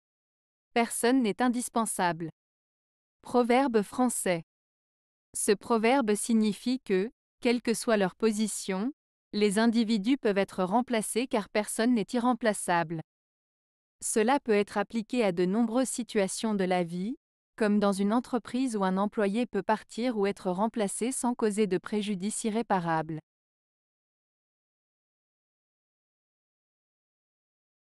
Personne n'est indispensable. - Proverbe Français
Ce proverbe signifie que, quelle que soit leur position, les individus peuvent être remplacés car personne n'est irremplaçable. Cela peut être appliqué à de nombreuses situations de la vie, comme dans une entreprise où un employé peut partir ou être remplacé sans causer de préjudice irréparable.